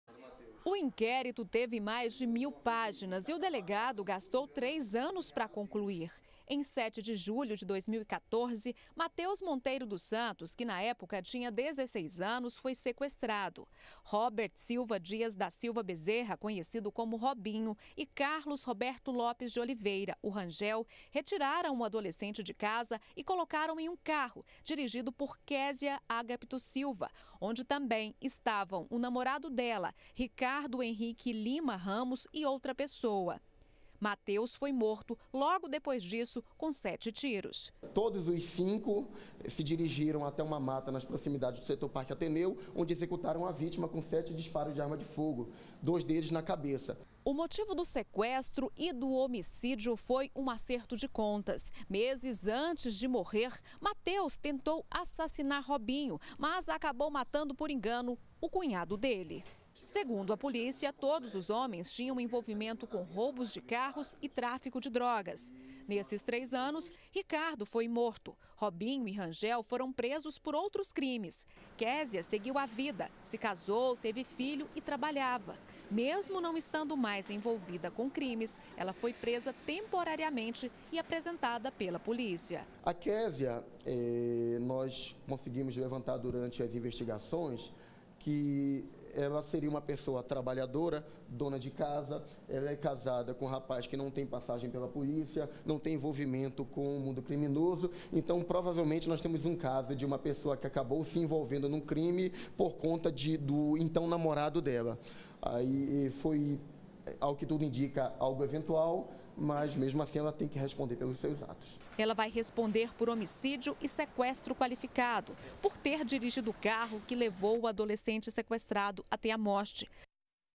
BG - Polícia conclui inquérito sobre assassinato - 17-10-2017
A polícia concluiu o inquérito sobre a morte de Matheus Monteiro dos Santos em julho de 2014. Na época do crime, a vítima tinha 16 anos de idade. Hoje uma mulher que participou do crime foi apresentada.